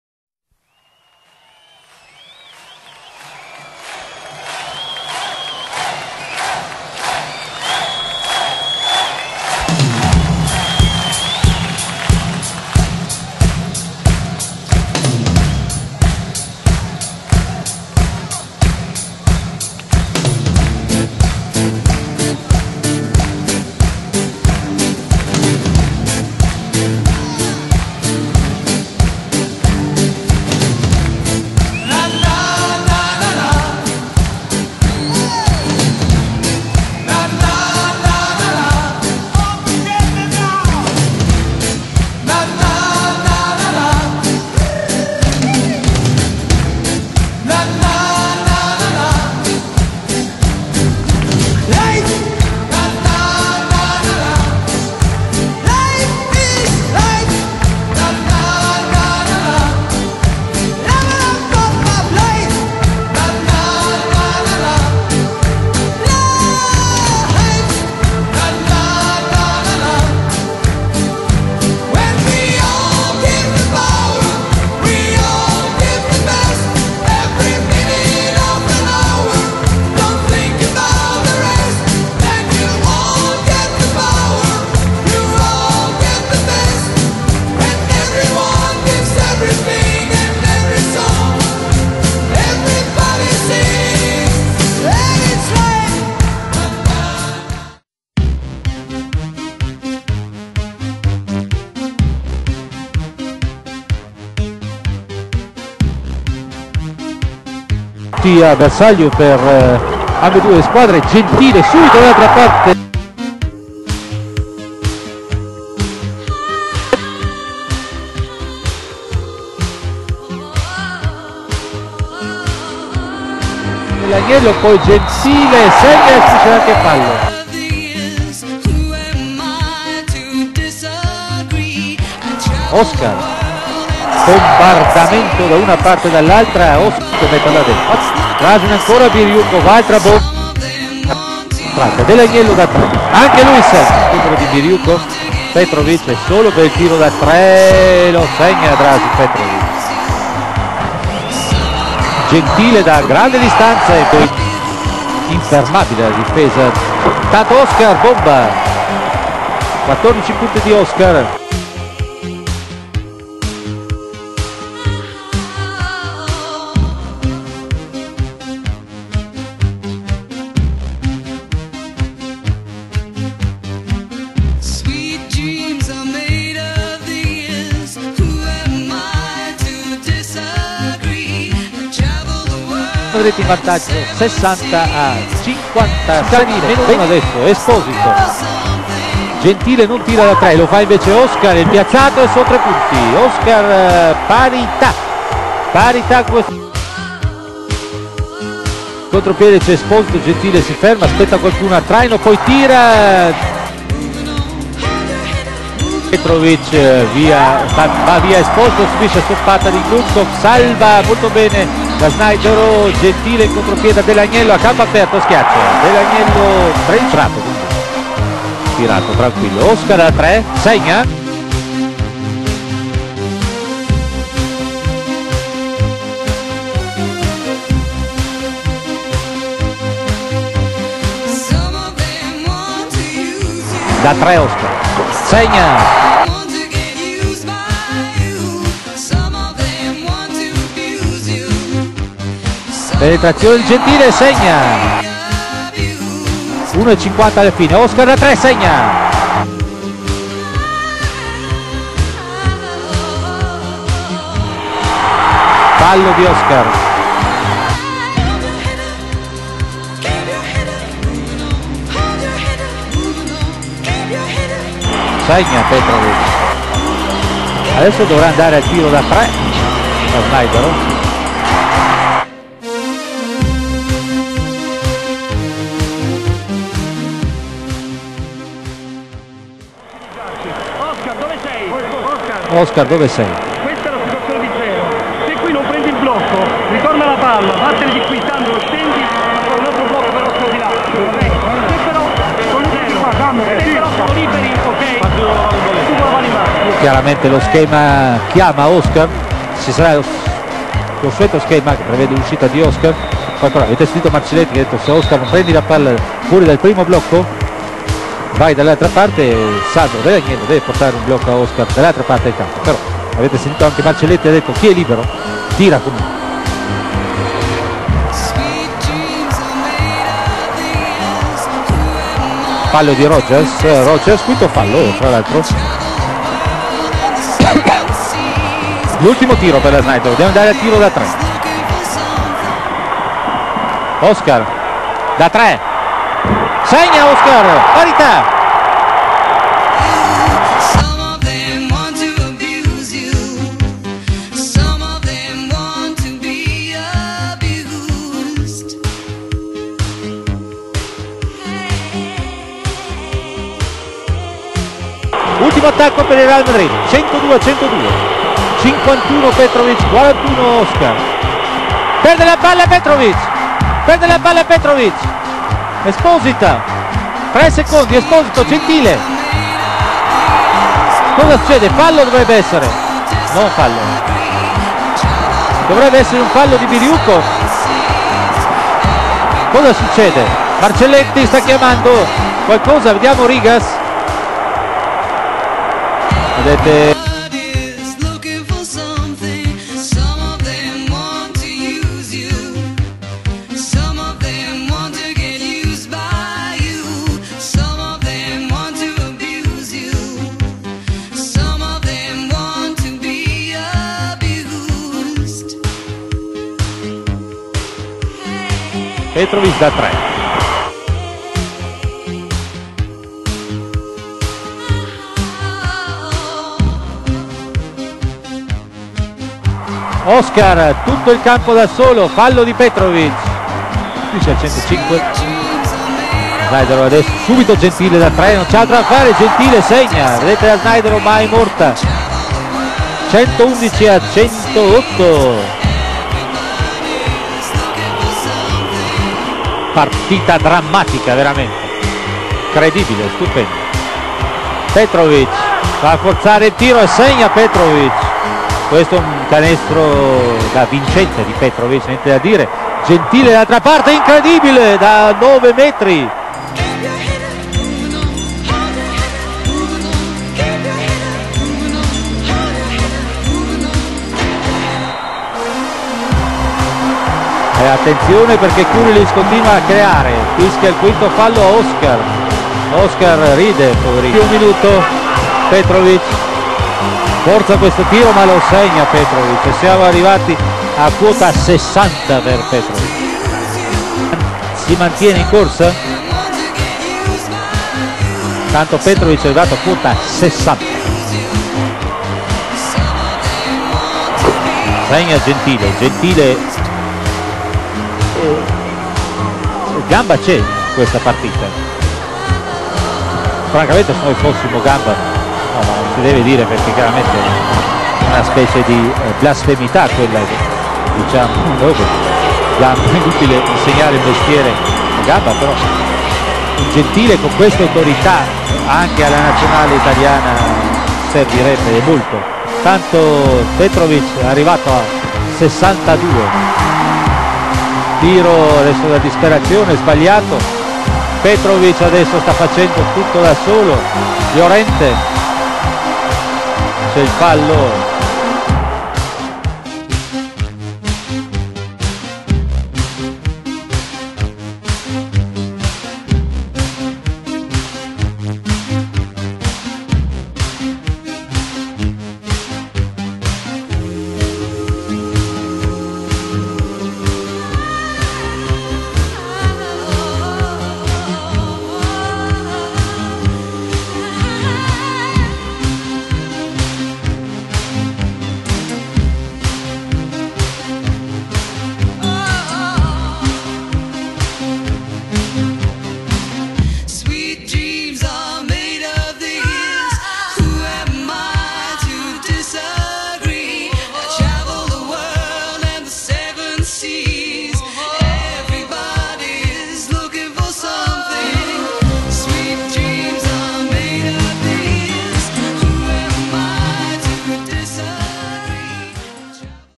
Juvecaserta - Atene 14 marzo 1989
Finale Coppa delle Coppe
Atene 14 Marzo 1989
Snaidero Caserta - Real Madrid